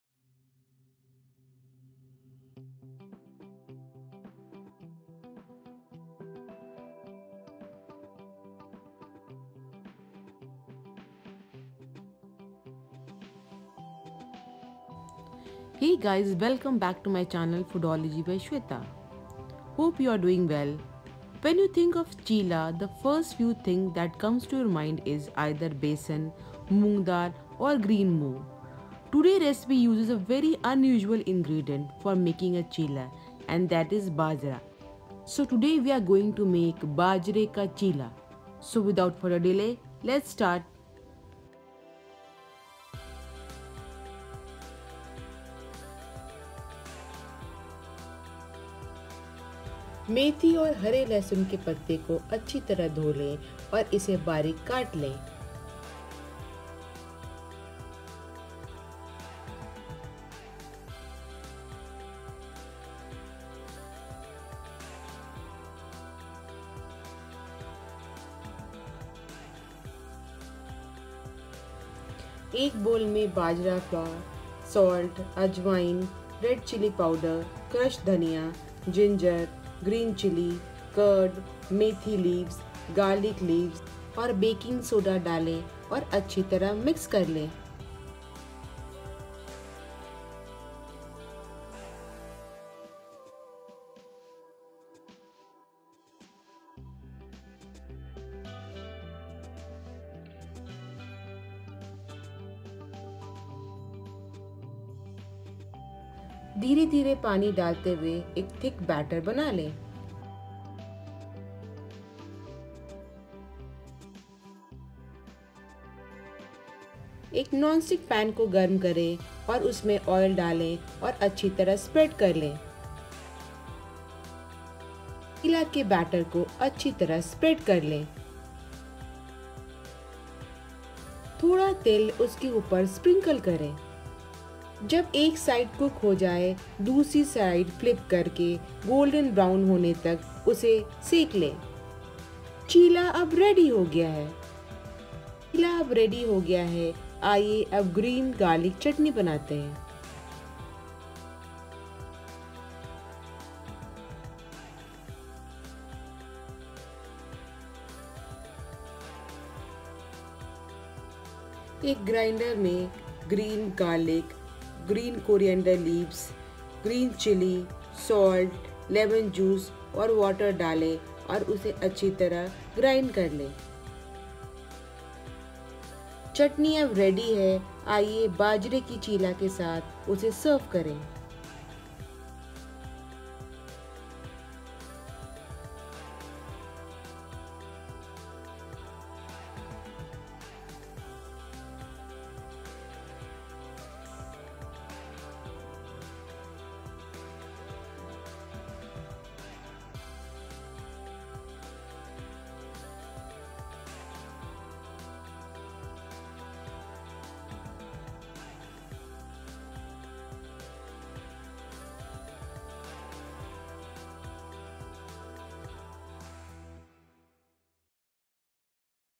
Bajra Cheela Recipe | Pearl Millet Pancake | Gluten-Free Recipe
Ingredients
1 cup Pearl Millet Flour ( Bajra)1 tbsp Ginger Garlic Green Chillies Paste1 tsp Carom Seeds (untoasted)1 tbsp Red Chilli Powder1 tbsp Sesame Seeds (untoasted)1/2 cup Dahi

Bajra Methi Chilla is a healthy breakfast option that you should try. These yummy chillas are made by combining Bajra Flour with methi leaves along with other spices. These are very easy to make and are a perfect option to pack for a lunch box or enjoy breakfast. The icing on the cake is that Bajra is a gluten-free grain and so is perfect for those who are gluten intolerant. These chillas are very quick and easy to make and you just need to mix up all the ingredients and make chillas.